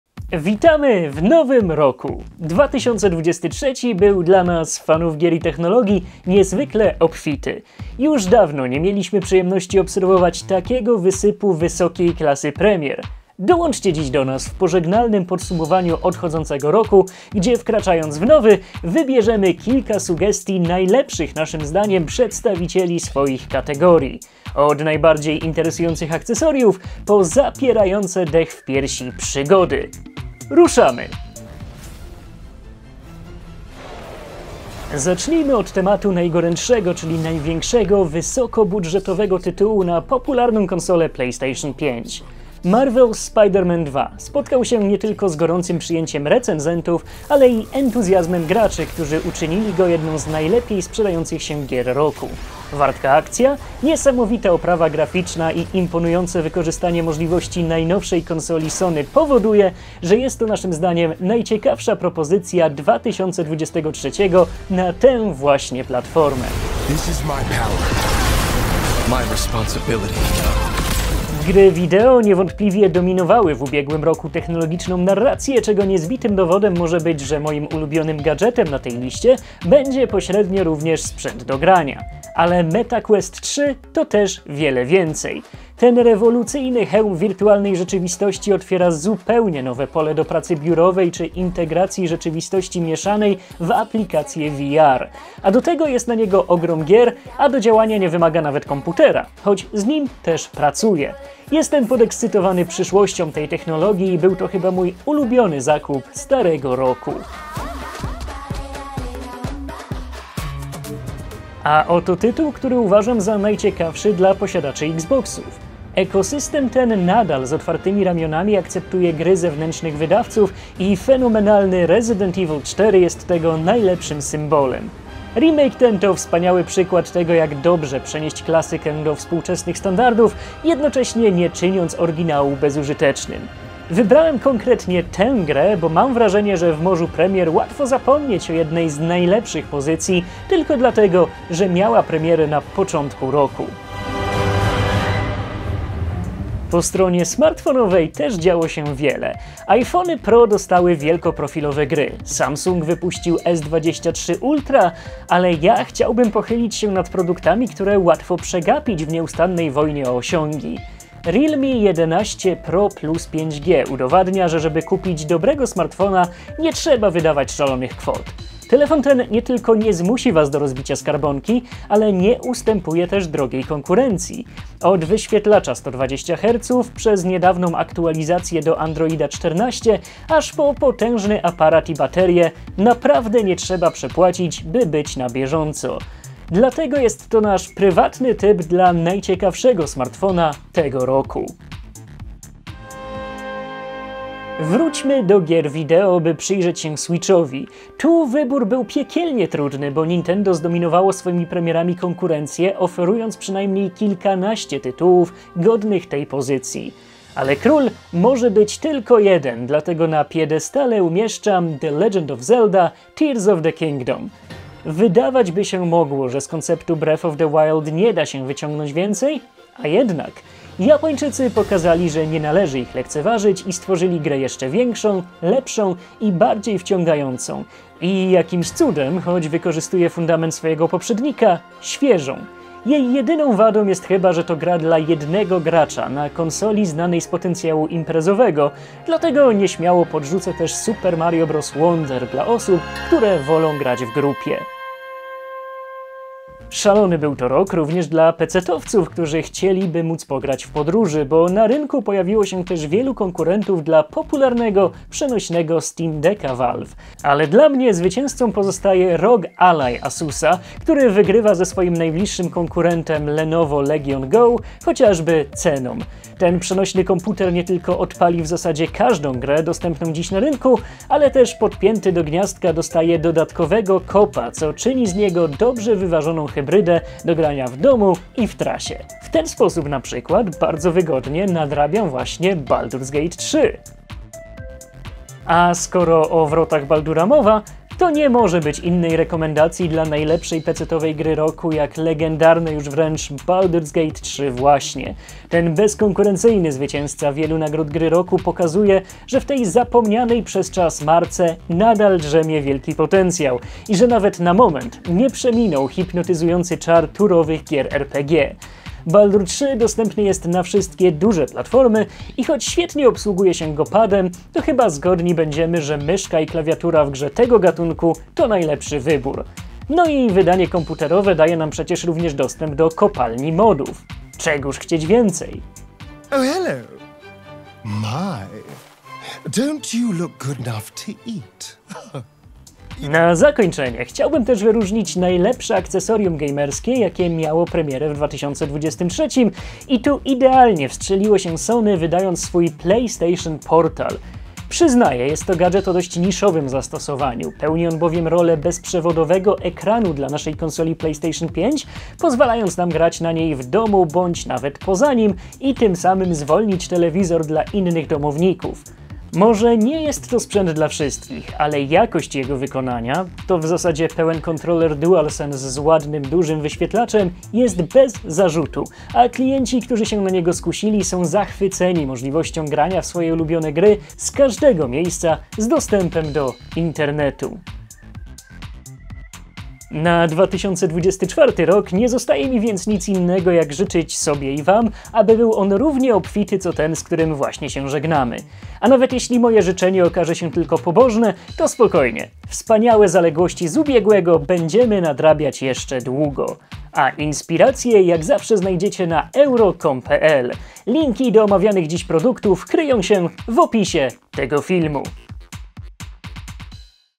🎥🌟 Podsumowanie roku 2023 według arhn.eu – RTV EURO AGD 🌟🎥
Rok 2023 obfitował w wiele interesujących premier dla fanów gier i technologii! 🚀🎮  Arek Kamiński, redaktor naczelny arhn.eu, przedstawia gry i gadżety, które zadebiutowały w minionym roku i zasłużyły na szczególne wyróżnienie.

Nie wiesz jaki produkt wybrać? Pomożemy Ci!